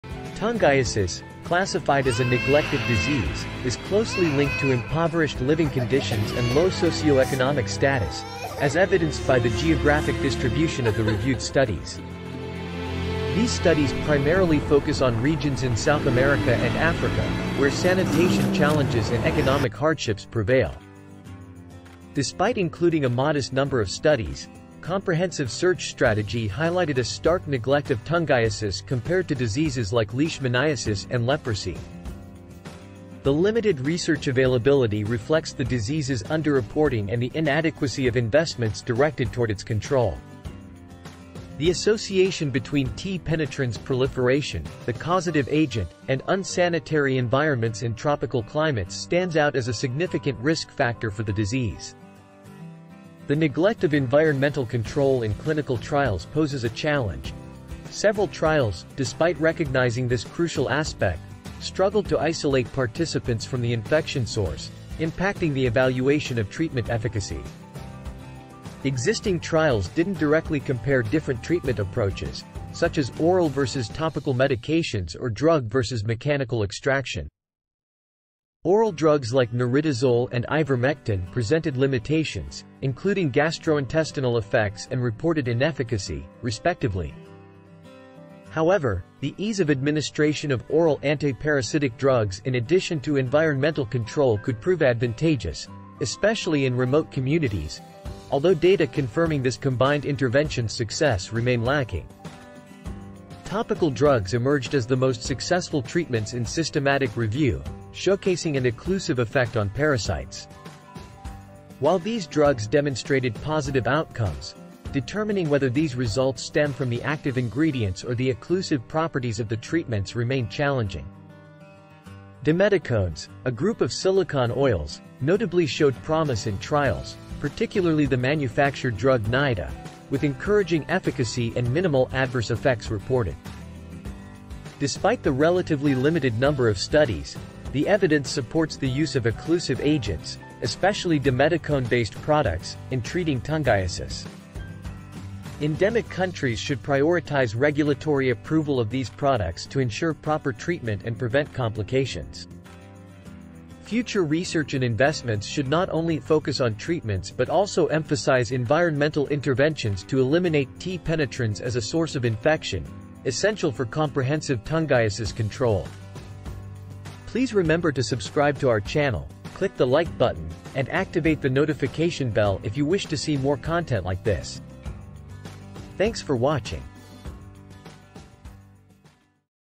Neglected Disease Tungiasis: Impact, Treatment Insights, and Urgent Need for Environmental Control
Tungiasis, classified as a neglected disease, is closely linked to impoverished living conditions and low socioeconomic status, as evidenced by the geographic distribution of the reviewed studies. These studies primarily focus on regions in South America and Africa, where sanitation challenges and economic hardships prevail. Despite including a modest number of studies, comprehensive search strategy highlighted a stark neglect of tungiasis compared to diseases like leishmaniasis and leprosy.